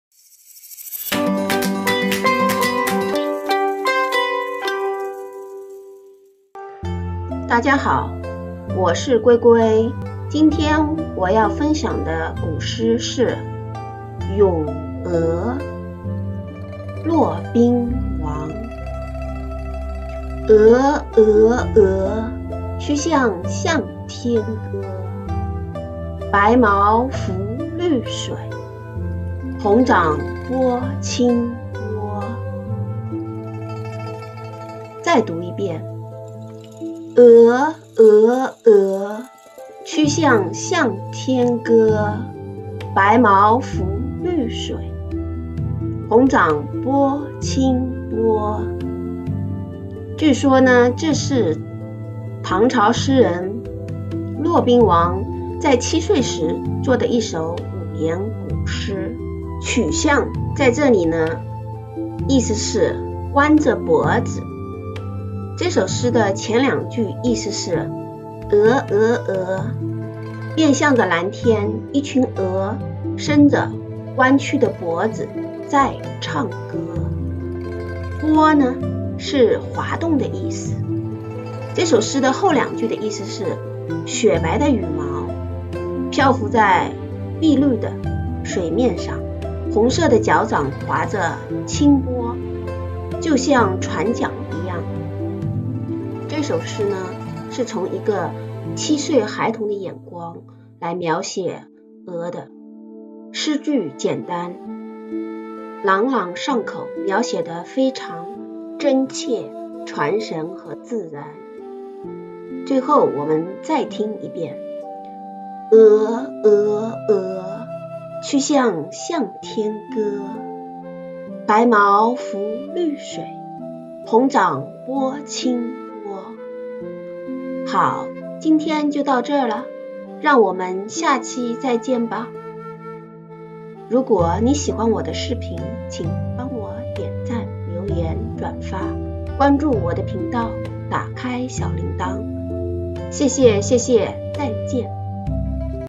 第36期 唐诗三百首，咏鹅，骆宾王，唐诗赏析，Tang poetry，subtitles
龟龟一直很喜欢唐诗宋词，希望通过唐诗宋词传递中华传统文化，可以让跟多的人喜欢唐诗宋词！
素材取自唐诗三百首和网络。
儿童故事:
狼和七只小山羊